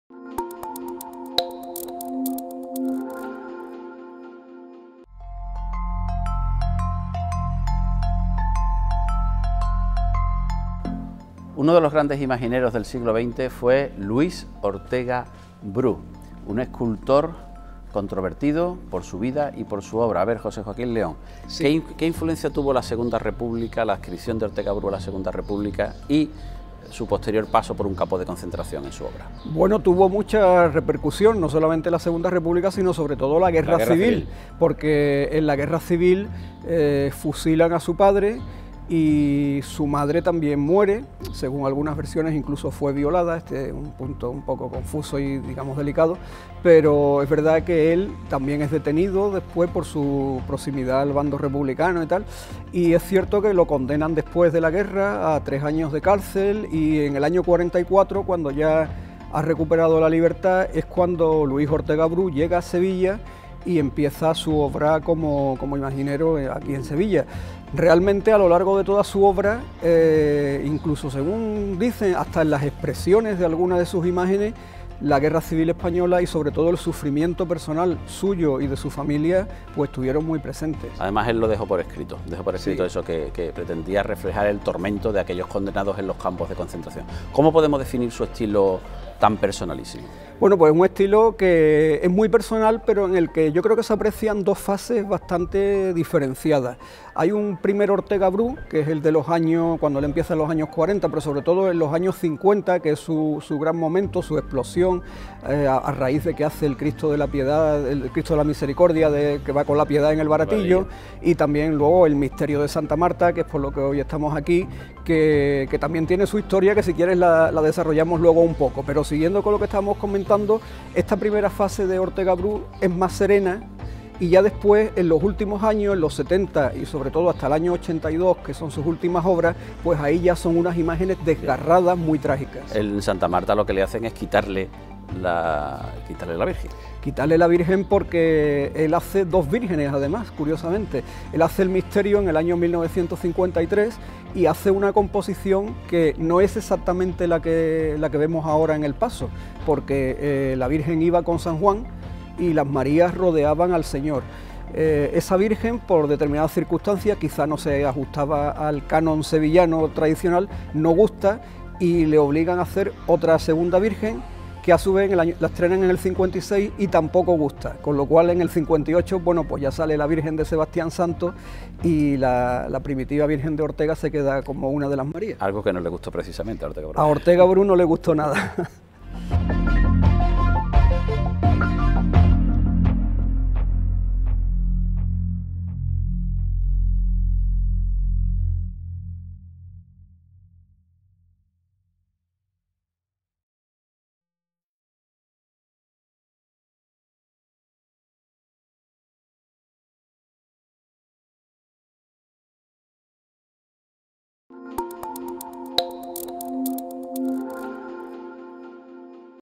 10 | La Cultura está en la calle | Luis Ortega Bru | José Joaquín León y Paco Robles
Hoy “Luis Ortega Bru” con José Joaquín León, escritor y periodista en la serie “La cultura está en la calle” de la Fundación San Pablo Andalucía CEU dirigida por el escritor y profesor de la Vniversitas Senioribus Francisco Robles, que ha contado con la colaboración de la Obra Social de la Caixa.

Vniversitas Senioribus, el aula de mayores de CEU Andalucía, utilizará este material audiovisual para el taller: “La cultura está en la calle”, que se celebrará durante el curso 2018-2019 en este centro cultural dedicado a los mayores sevillanos que quieren disfrutar aprendiendo.

tfno.954488070